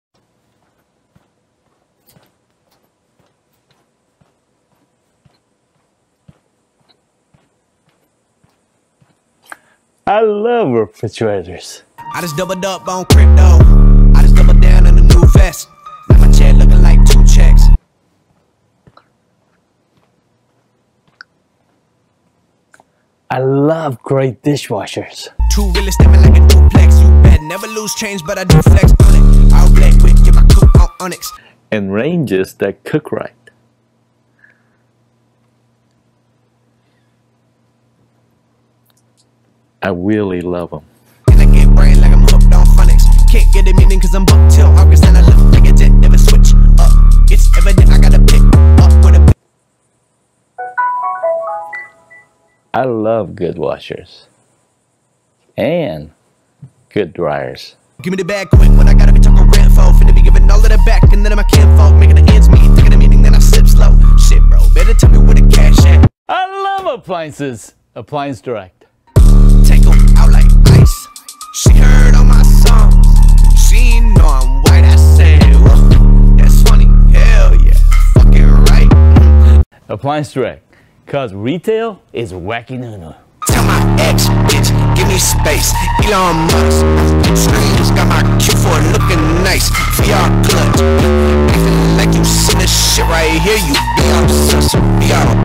I Love Appliances // Wakkie Nu Nu Edition
I was trying to make some breaks for my upcoming live streams, and came up with this monstrosity. I thought it would make a funny meme video. It's definitely out of the blue. You may have seen the "I Love Refrigerators" video, but did you know my boy, Sam Pak, also loves other appliances including dishwashers, ranges, washers, AND dryers?? Well, now you know, son.